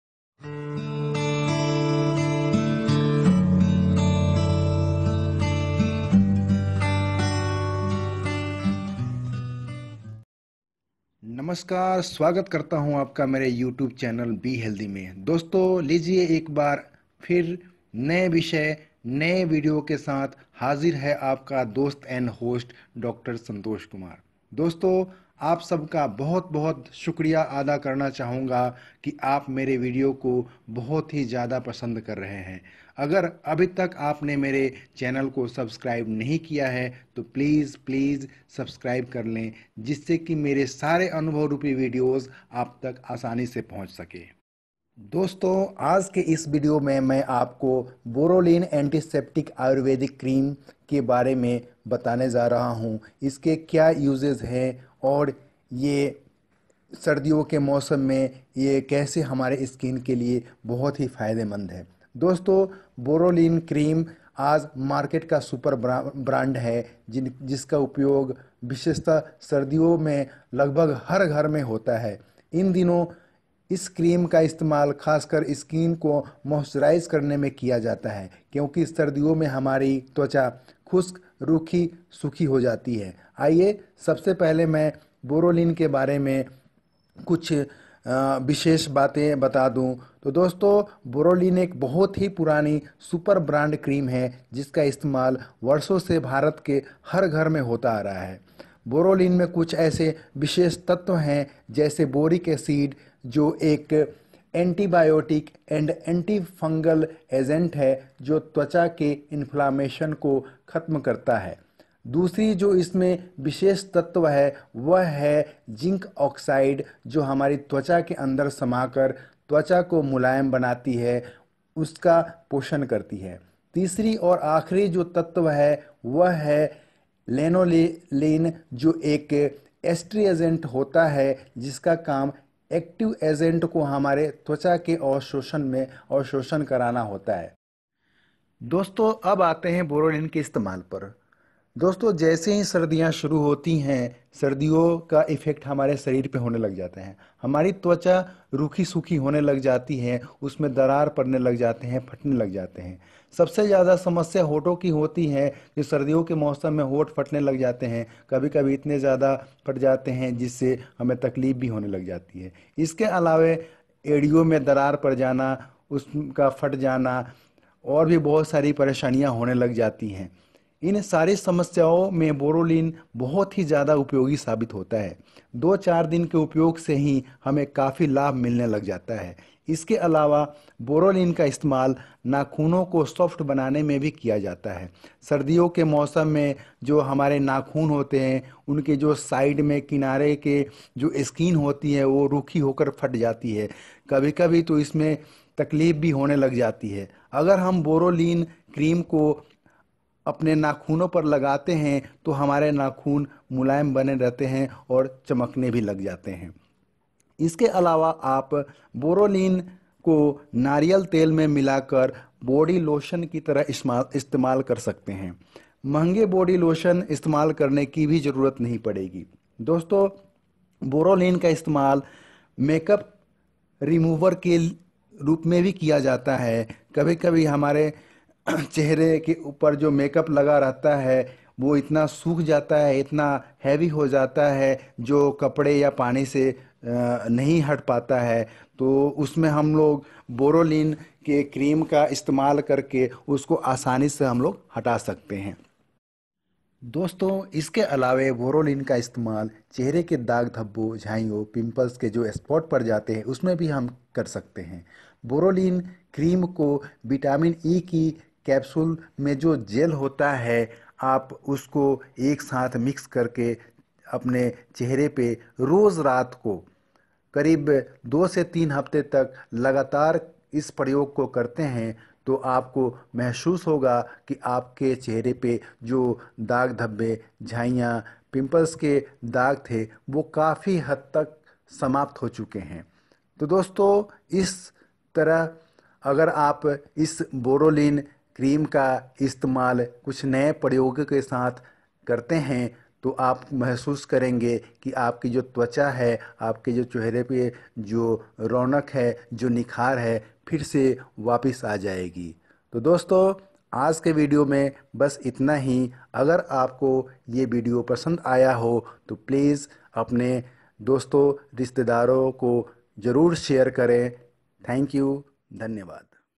ऐसे करें बोरोलीन का इस्तेमाल पाएं त्वचा बेदाग चेहरे पर निखार। How to use Borolene anticeptic cream
नमस्कार दोस्तों।
       बोरोलीन किसी परिचय का मोहताज नहीं है।भारत के हर घर में वर्षो से इसका इस्तेमाल होता आ रहा है यह भारत का सुपरब्रांड प्रोडक्ट्स है। आज मैं आपको इसके सामान्य प्रयोग के अलावे कुछ विशेष प्रकार के इस्तेमाल के बारे में मेरे इस वीडियो में बता रहा हूँ इस वीडियो को आपको अवश्य देखना चाहिए।